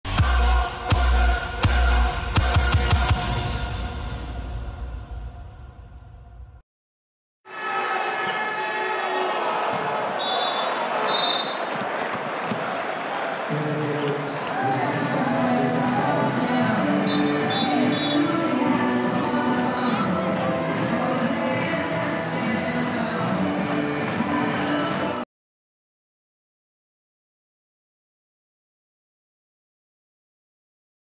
Dean Bombac - mojstrovina
Povratnik v slovensko rokometno reprezentanco, Dean Bombač je v sredo, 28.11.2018 ponovno navdušil in spomnil na rokometnega Luko Dončiča. Izkazal se je namreč z izjemno potezo na včerajšnji tekmi EHF Lige prvakov, ko je Pick gostoval pri Flensburgu, a izgubil z rezultatom 25:27.